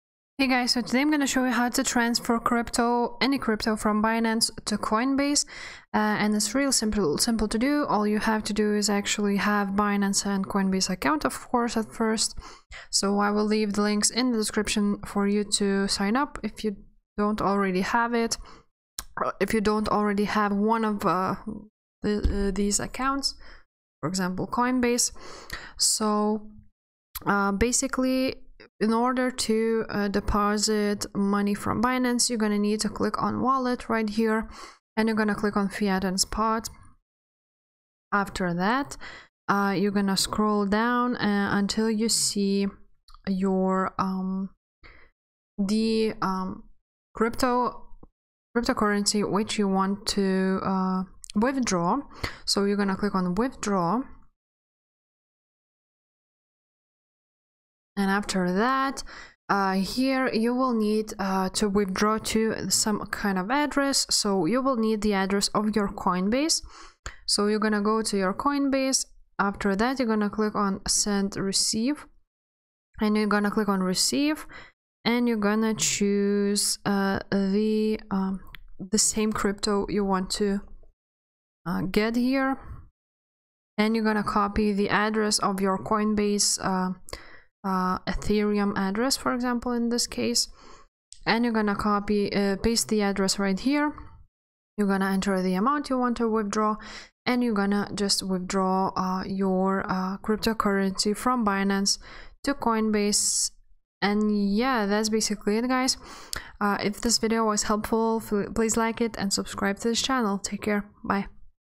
How To Transfer Crypto From Binance To Coinbase
In this video I will show you how to transfer crypto from Binance to Coinbase!